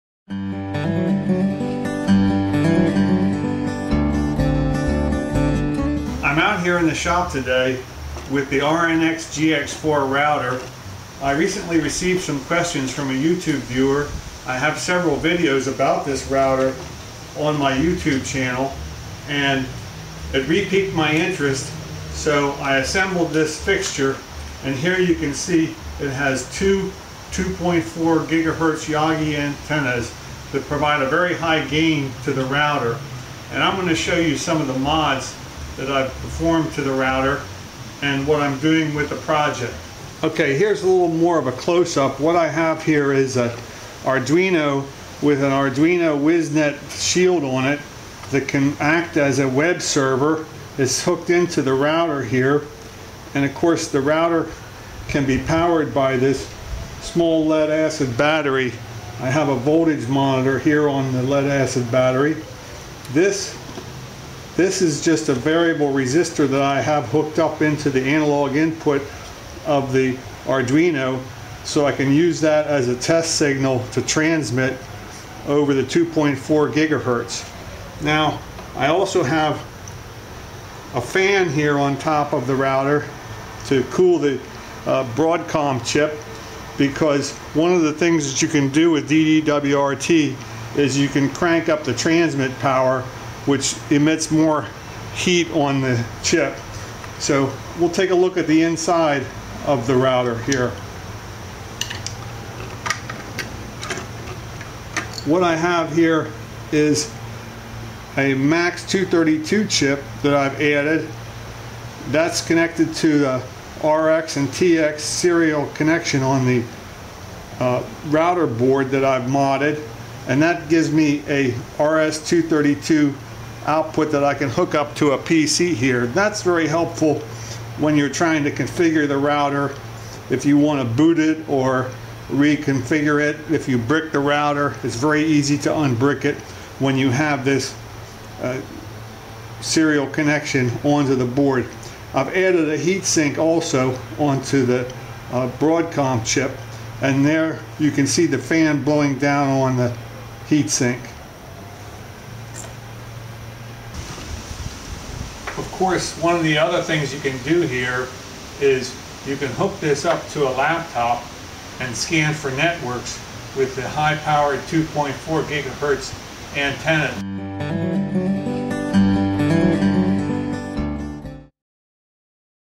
Wardriving Arduino Wiznet Webserver High Power Wireless
This project uses the RNX-GX4 router configured with DD-WRT connected to an Arduino Wiznet Board as a webserver.